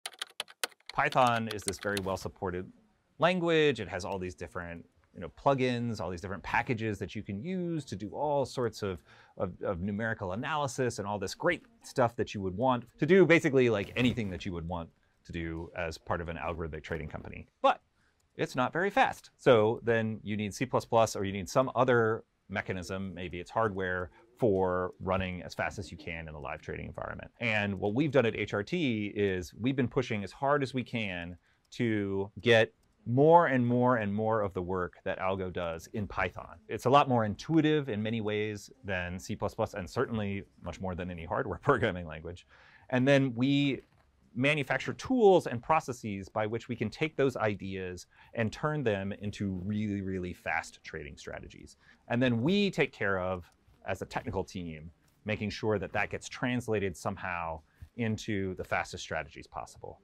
How HRT uses Python 🐍
How Hudson River Trading Uses Python Language

While C++ coaxes every bit of performance out of our trading infrastructure, modern Python allows us to research, build, and adapt systems to new requirements as fast as we can think. 💭

We sat down with Bill Vanderson, head of Python@HRT, to discuss how HRT uses Python to optimize developer efficiency and throughput. Besides providing increased productivity, Python provides tools like notebooks and a great data stack for research, structured concurrency for building robust async systems (trio!), a great testing story (pytest!), interoperability with other languages, and an awesome community.

Follow HRT!